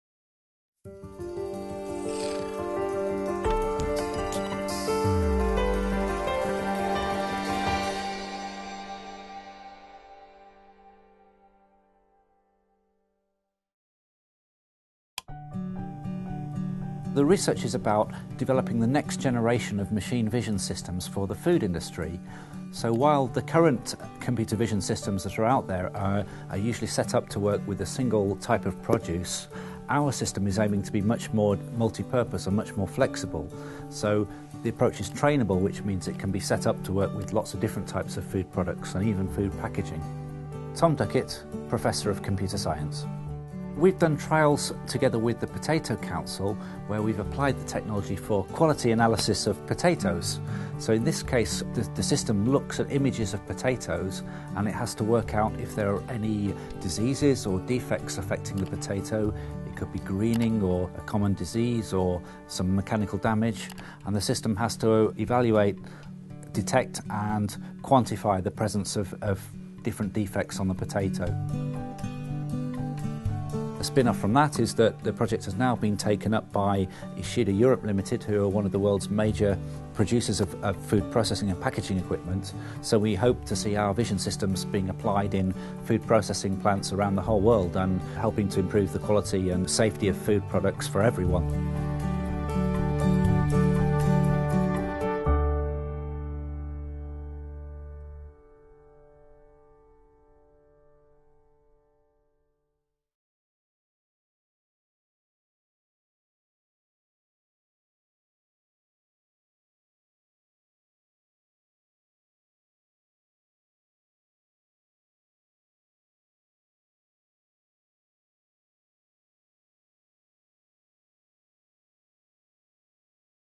Research Showcase: Trainable Visual Anomaly Detection & Diagnosis | University of Lincoln
University of Lincoln Research Showcase: Visual Anomaly in the Food Industry
Professor Tom Duckett, School of Computer Science

We expect the food we buy to be free from disease and defects. Yet the computer systems that make this possible are extremely complex. Prof Tom Duckett is developing the next generation of machine vision systems for the food industry, creating a programme that is more flexible and multi-purpose.
📍 Address: University of Lincoln, Brayford Pool, Lincoln, LN6 7TS

After carrying out trials with the UK Potato Council, where the system looks at images of potatoes and 'learns' how to spot blemishes and defects, the project has now been taken up by one of the world's largest producers of food processing and packaging equipment.